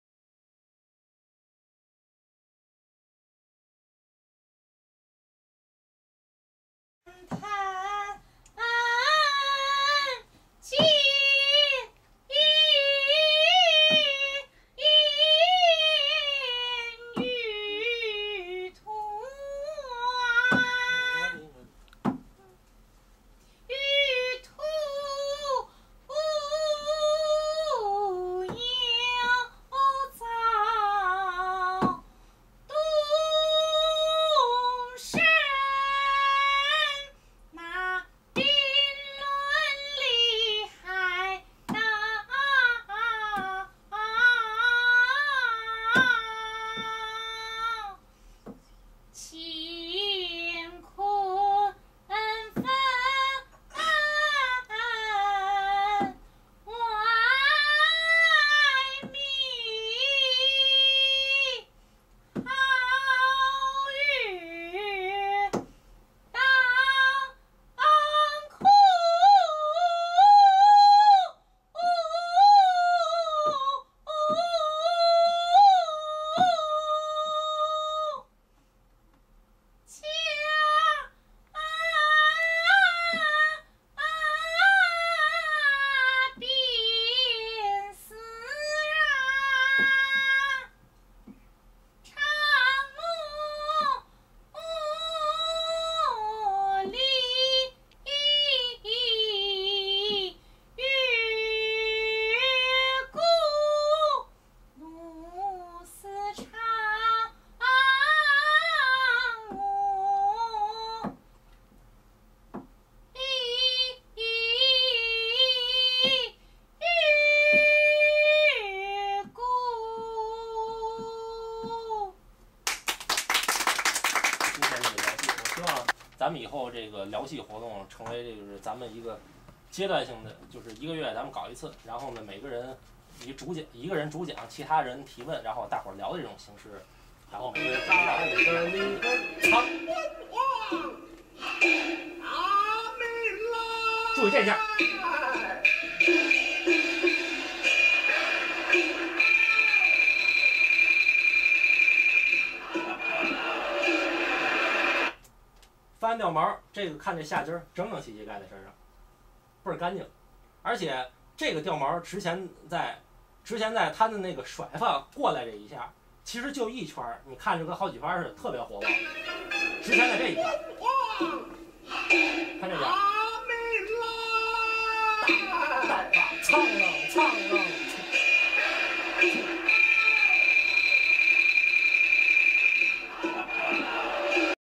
紐約梨園社青年團京劇賞析小講座 NYCOS YouthTroupe Opera Discussion 09_3_12
NYCOS' Youth Troupe members having a small discussion on the singing, movements, and presentation of Peking Opera. Look out for NYCOS' September 11 performance at CenterLight Center Chinatown this year. Twitter @NYCOS1 on Twitter and Weibo

紐約梨園社青年團成員舉行了京劇賞析小講座，以後將會舉辦一系列的小講座。紐約梨園社將於2012年9月11日上午11AM于唐人街CenterLight老人中心演出。更多詳情請瀏覽 Twitter @NYCOS1 on Twitter 以及微博@紐約梨園社